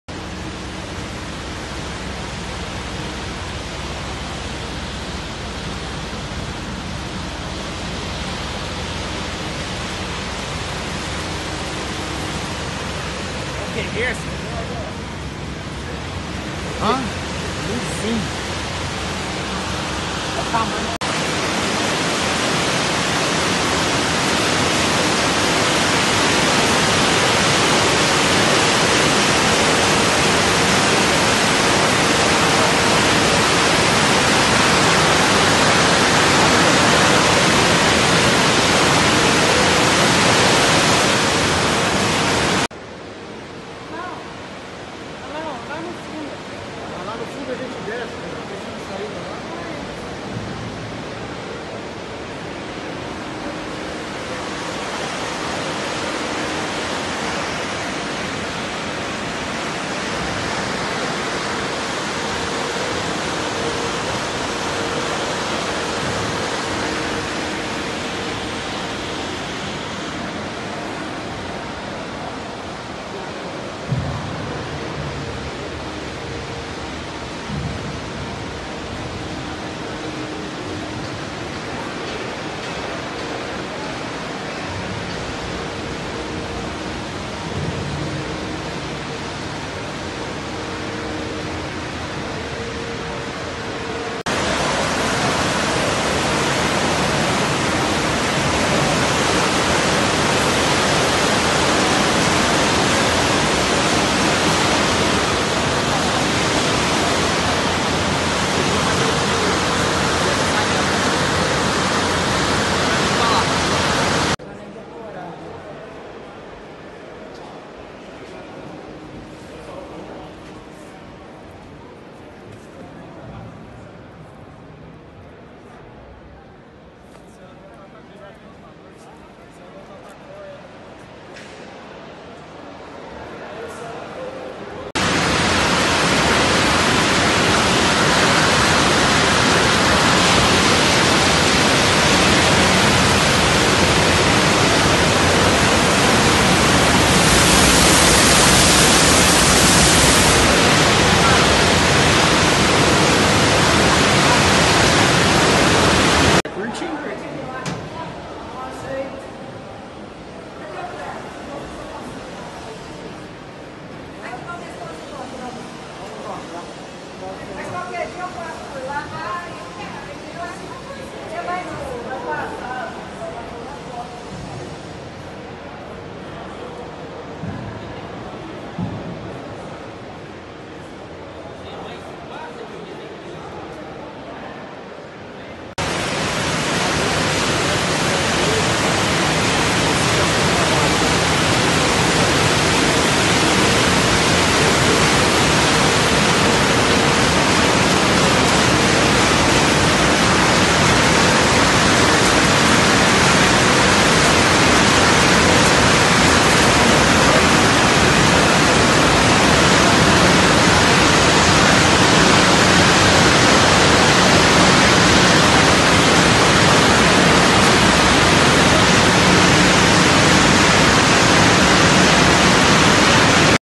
Conheça a nova estrutura de processamento pós colheita de café da Fazenda
Visita ao novo complexo de pós colheita de café da Ipanema Coffees no Sul de Minas